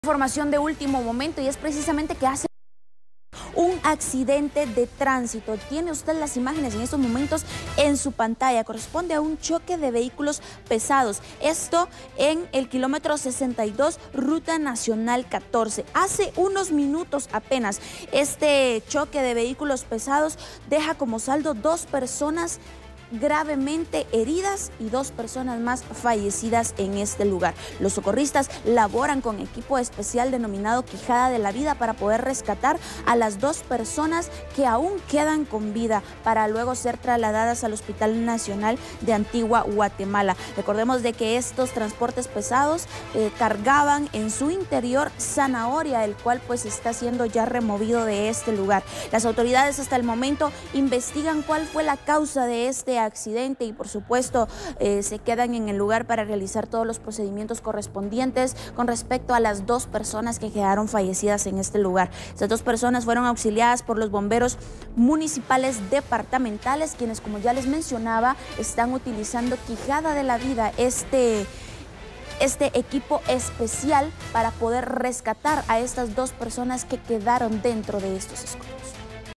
Accidente de tránsito en la ruta Nacional 14
Un accidente de tránsito ocurrió en el kilómetro 62 de la ruta Nacional 14. Un bus y un camión chocaron, dejando dos muertos y dos heridos.